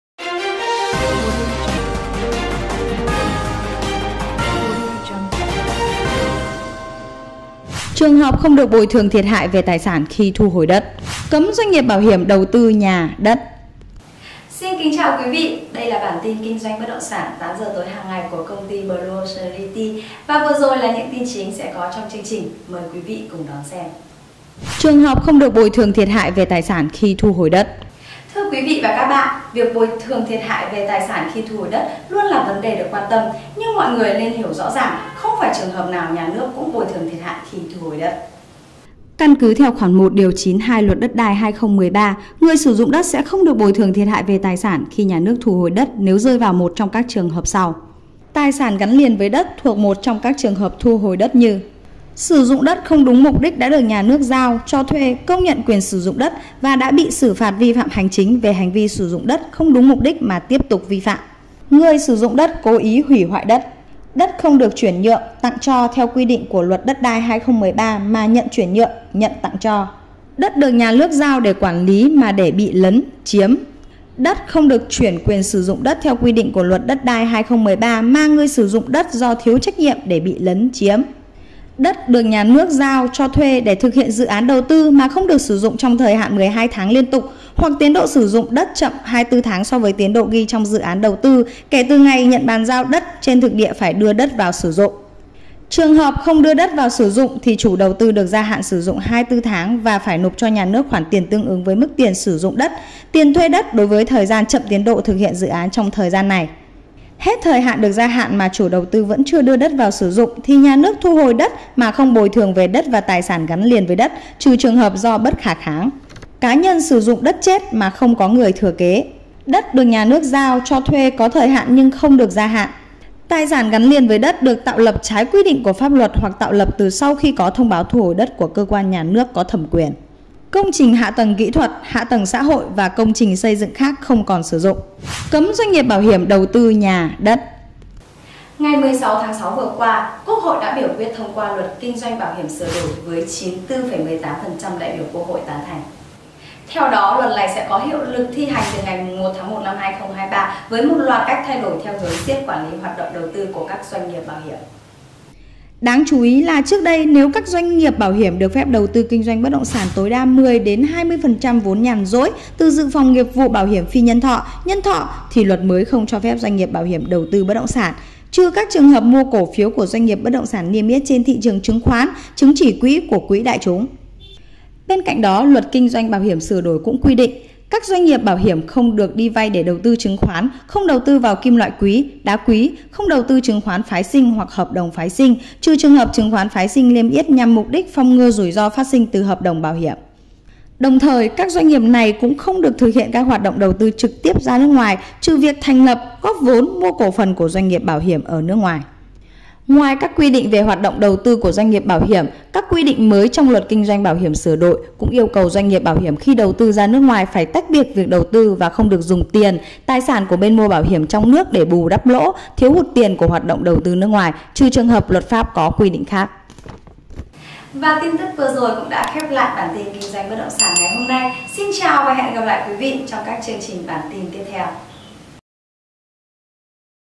Trường hợp không được bồi thường thiệt hại tài sản khi thu hồi đất | Cấm bảo hiểm đầu tư nhà, đất
Trường hợp không được bồi thường thiệt hại tài sản khi thu hồi đất
Thưa quý vị và các bạn việc bồi thường thiệt hại về tài sản khu thu hồi đất luôn là vấn đề được quan tâm, nhưng mọi người cần hiểu rõ ràng không phải trường hợp nào cũng được nhà nước bồi thường thiệt hại khi thu hồi đất.
Cấm doanh nghiệp bảo hiểm đầu tư nhà, đất
Ngày 16/6 vừa qua,  Quốc hội đã biểu quyết thông qua Luật Kinh doanh bảo hiểm sửa đổi với 94,18% đại biểu quốc hôi tán thành. Theo đó, luật này sẽ có hiệu lực thi hành từ ngày 1/1/2023 với một loạt thay đổi theo hướng siết quản lý hoạt động đầu tư của các doanh nghiệp bảo hiểm.
👉Blue Ocean Realty - Kênh Youtube cập nhật tin tức bất động sản uy tín tại Việt Nam
Tư liệu nguồn tin được tổng hợp từ: tienphong.vn  và  laodong.vn